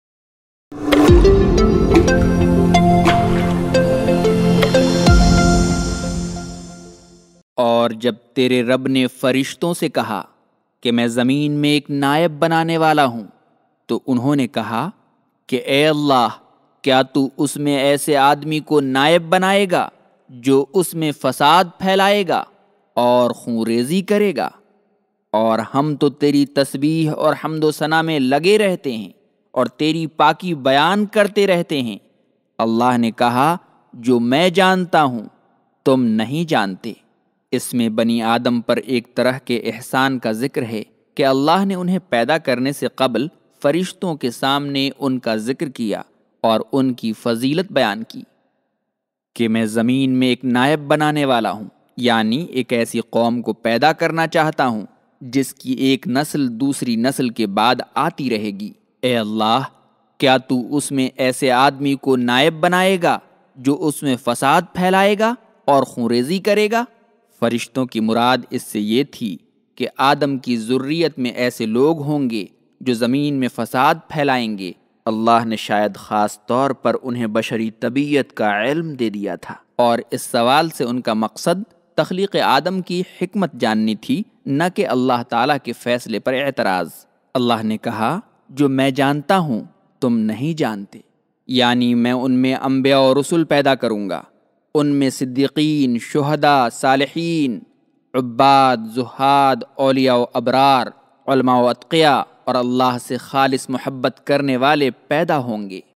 dars-e-quran Aayat no.30/ allah ne insaano ko apna khleefa banane ka faisla kia
اور جب تیرے رب نے فرشتوں سے کہاکہ میں زمین میں ایک نائب بنانے والا ہوں تو انہوں نے کہا کہ اے اللہ کیا تو اسمیں ایسے آدمی کو نائب بنائیگاجو اسمیں فساد پھیلائیگا اور خونریزی کریگا اور ہم تیری تسبیح اور حمد و ثنا میں لگے رہتے ہیں اور تیری پاکی بیان کرتے رہتے ہیں اللہ نے کہا جو میں جانتا ہوں تم نہی جانتے اس میں بنی آدم پر ایک طرح کے احسان کا ذکر ہے کہ اللہ نے انہیں پیدا کرنے سے قبل فرشتوں کے سامنے انکا ذکر کیا اور انکی فضیلت بیان کی کہ میں زمین میں ایک نائب بنانے والا ہوں یعنی ایک ایسے قوم کو پیدا کرنا چاہتا ہوں جسکی ایک نسل دوسری نسل کے بعد آتی رہیگی اے اللہ کیا تو اسمیں ایسے آدمی کو نائب بنائیگا جو اسمیں فساد پھیلائیگا اور خونریزی کریگا فرشتوں کی مراد اسمیں یہ تھی کہ آدم کی ذریت میں ایسے لوگ ہونگے جو زمین میں فساد پھیلائنگے اللہ نے شاید خاص طور پرانہیں بشری طبعیت کا علم دے دیا تھا اور اس سوال سے انکا مقصد تخلیق آدم کی حکمت جاننی تھی نہ کہ اللہ تعالیٰ کے فیصلے پر اعتراض اللہ نے کہا جو میں جانتا ہوں تم نہی جانتے یعنی میں ان میں انبیاء و رسل پیدا کرونگا ان میں صدیقین شہداء صالحین عباد زھاد اولیاء و ابرار علماء و اتقیاء اور اللہ سے خالص محبت کرنے والے پیدا ہونگے